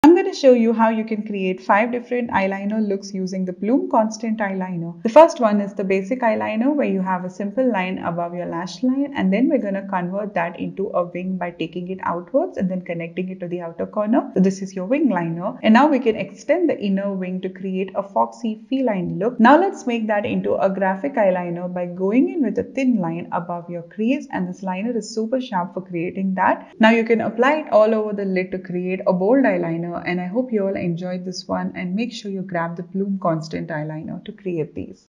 5 Eyeliner Styles To Rock At Any Occasion, & Any Outfit
Ready. Set. SERVE ✨
And Let Your Eyes Do The TALKING 🙌🏼💝

Basic, Winged, Foxy, Feline, Bold, tell us which one is your favorite eyeliner style in the comments below 👇🏻✅